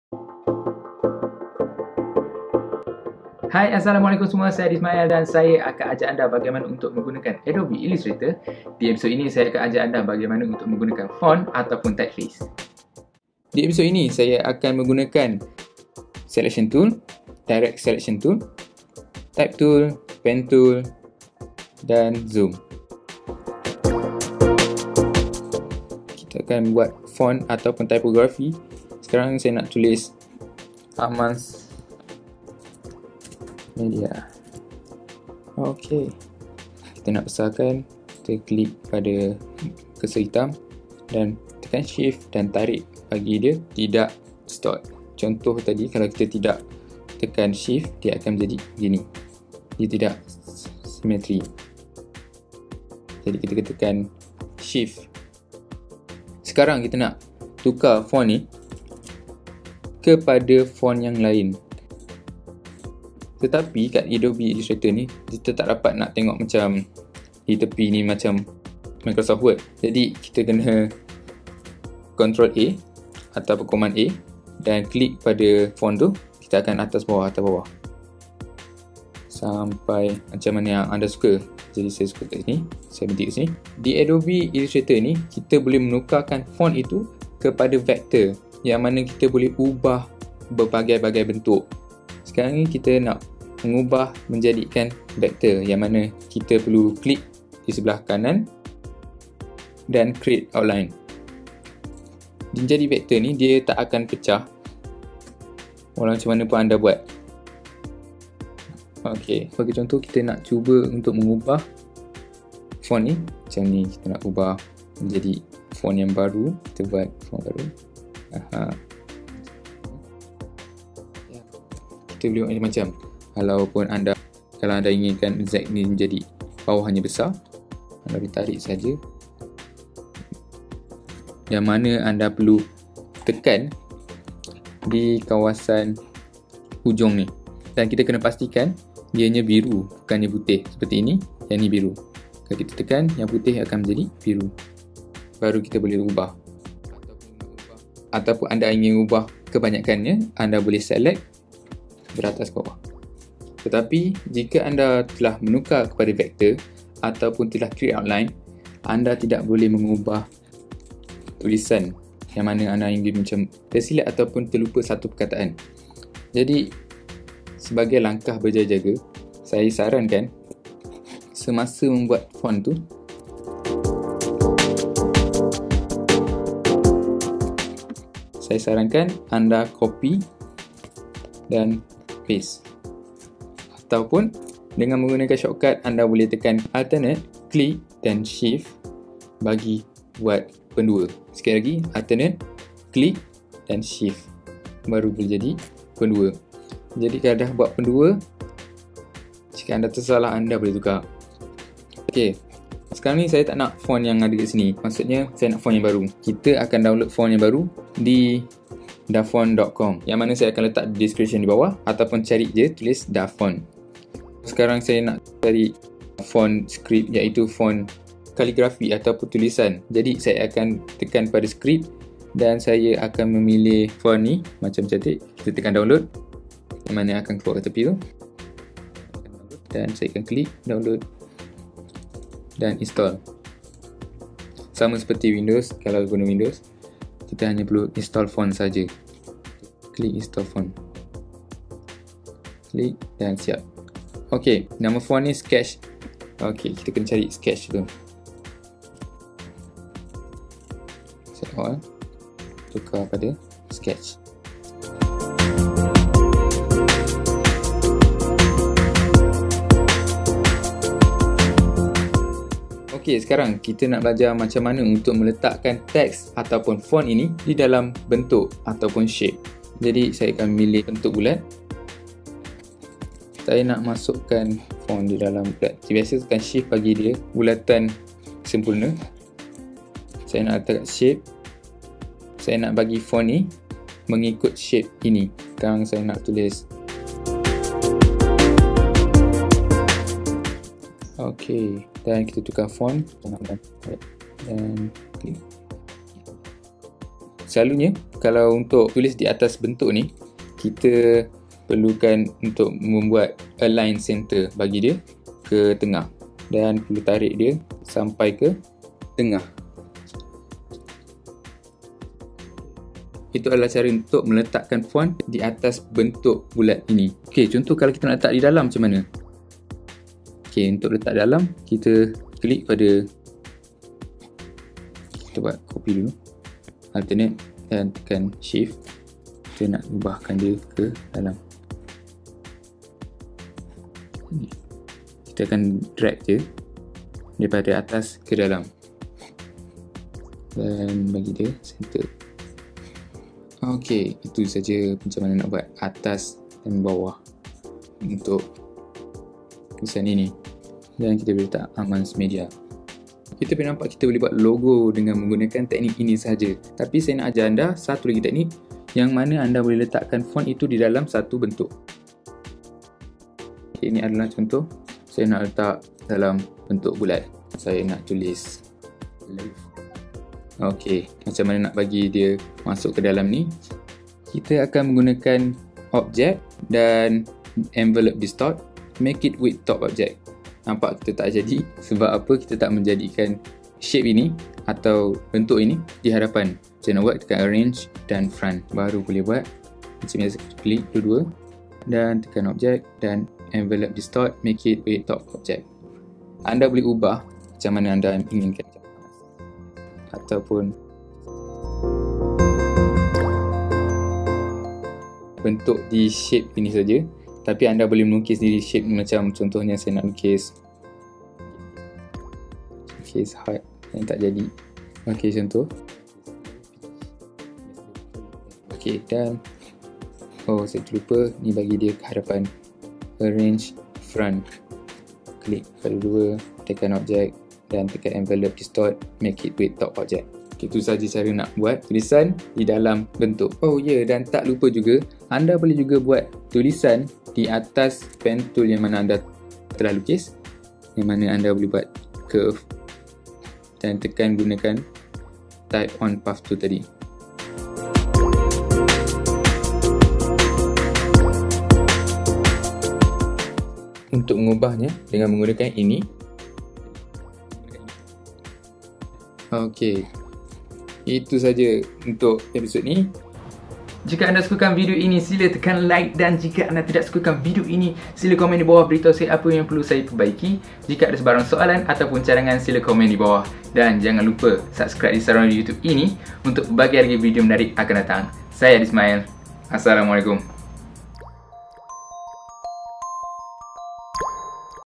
(Qibod) Adobe Illustrator - Belajar Menggunakan Muka-Taip
Video pertama dibawah siri Adobe Illustrator, dimana kita belajar secara ringkas bagaimana untuk menggunakan font atau muka-taip.

Qibod.com merupakan sebuah laman dibawah Amanz yang memfokuskan kepada penawaran video-video tutorial memfokuskan kepada kreatif multimedia dan juga pengaturcaraan.

Layari : Qibod.com | Belajar Secara Percuma